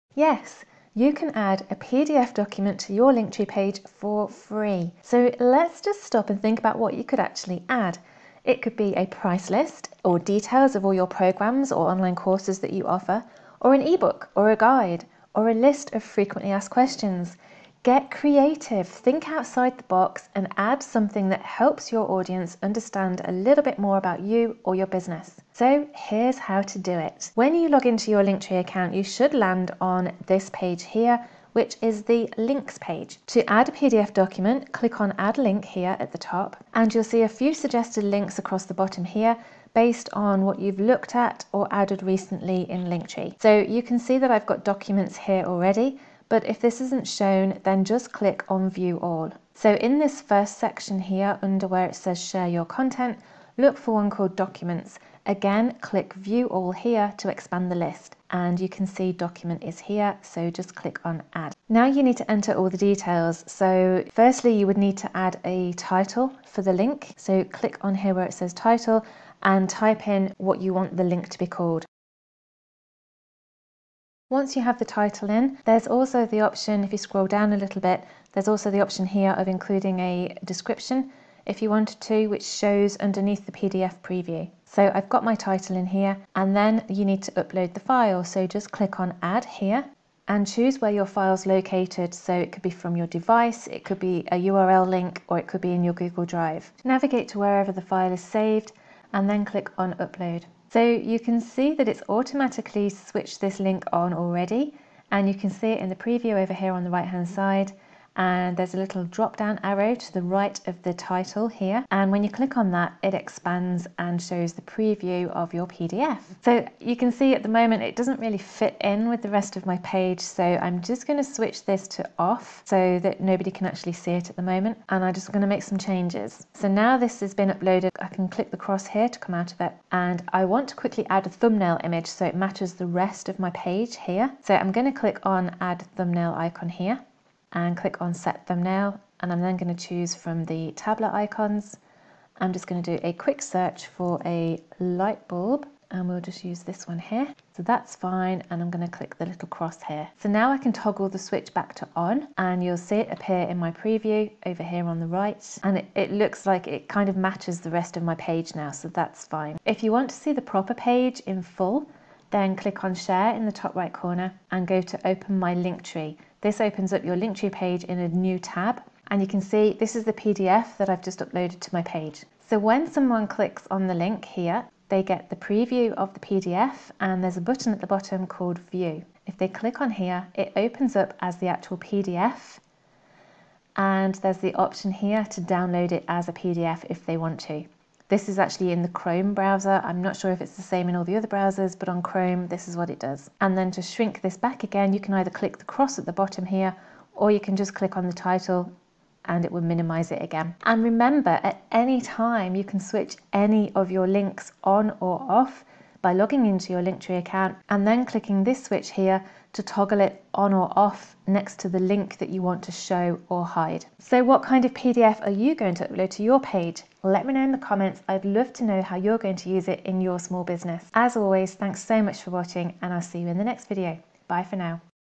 How To Add a PDF to Your Linktree Page For Free!
Adding a PDF document to your Linktree page is a handy way of helping your audience understand more about you or your business 🤓
And the good thing is you can do it with a FREE account!
What PDF are you going to add to your page?

✨ SIGN UP ✨
Join my growing community of Google Ninjas, where I share at least 1 actionable Google tip every 2 weeks

Sharon x​